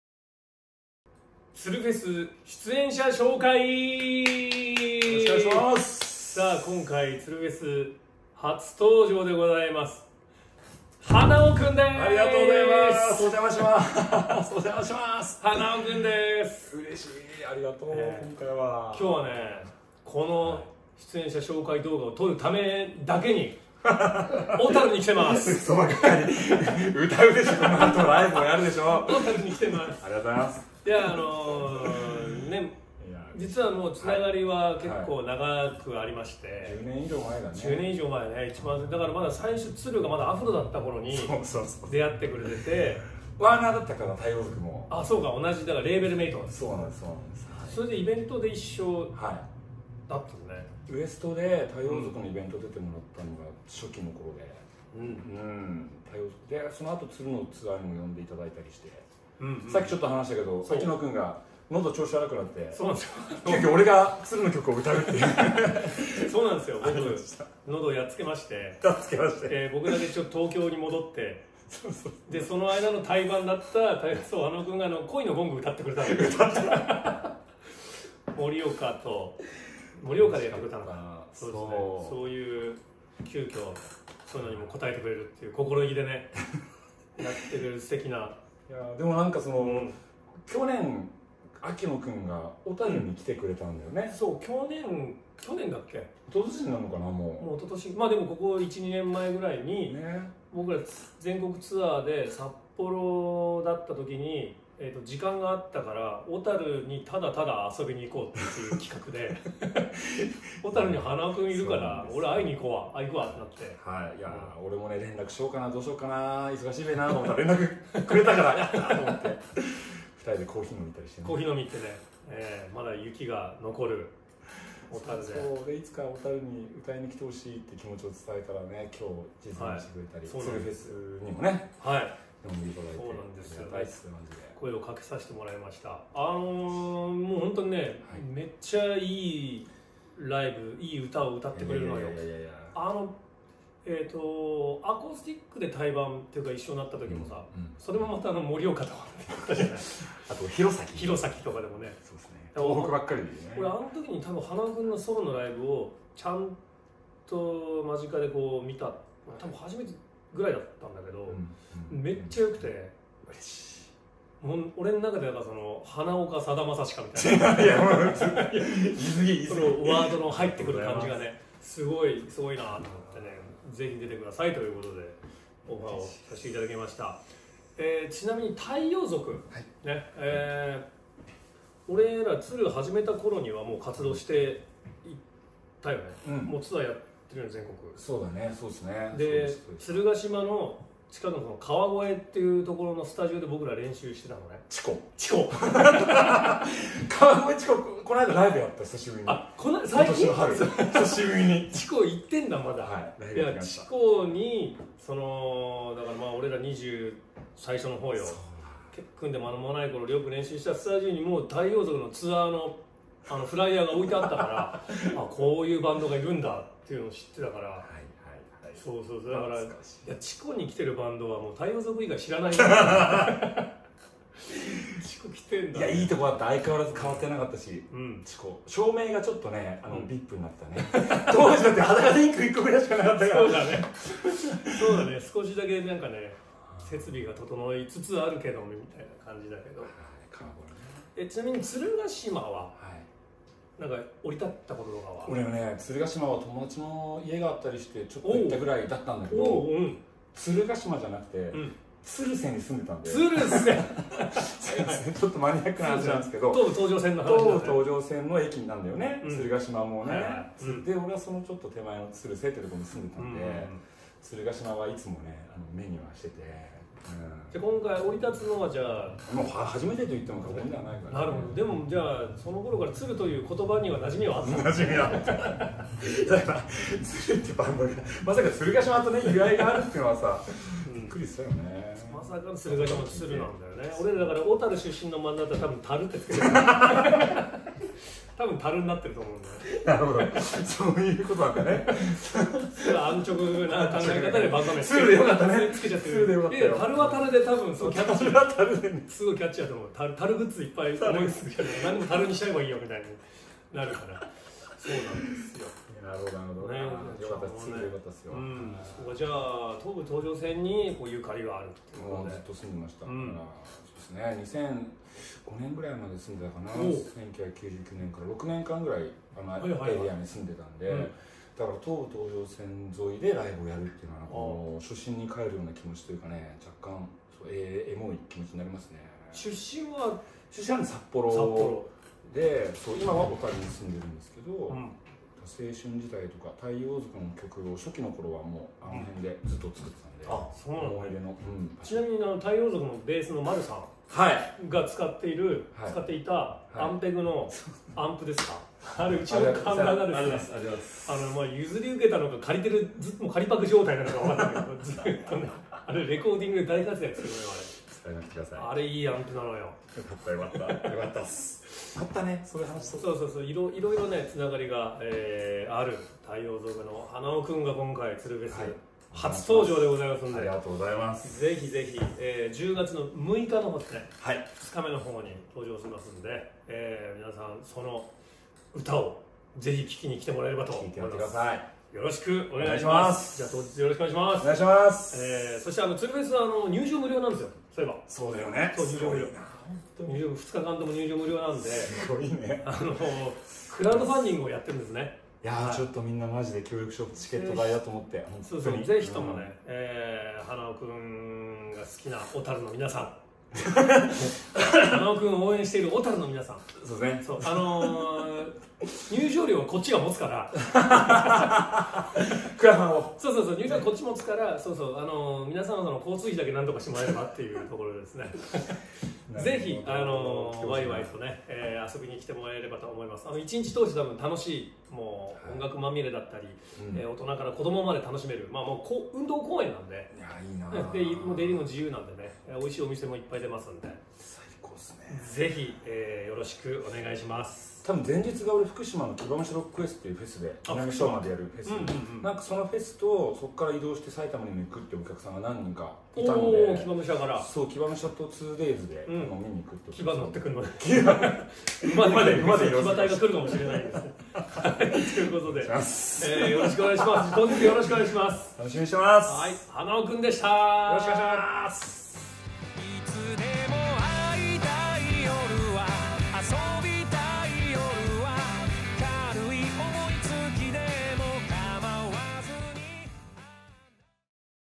鶴フェス2024出演アーティスト紹介／花男
鶴の地元、埼玉県鶴ヶ島市で開催する無料野外フェス
「鶴フェス2014 」DAY2・10/6(日)FOFTOOステージに出演してくれる
花男×鶴の対談動画です！

「一緒に盛り上げる全国のソウルメイト集合！“鶴フェス”応援大使大募集！」
クラウドファンディング実施中！
一緒に鶴フェスを2024を作りましょう。